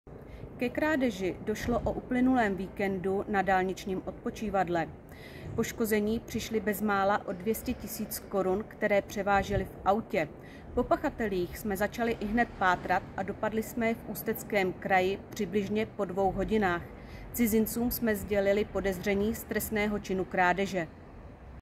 PELHŘIMOV: Krádež peněz
PELHŘIMOVSKO - HUMPOLECKO: Krádež peněz. Cizince, kteří se dopustili krádeže, policisté velmi rychle zadrželi. Dva cizinci ve věku třiatřicet a pětatřicet let, na dálničním odpočivadle ukradli skoro 200 tisíc korun... celý článek (video na webu) čtěte zde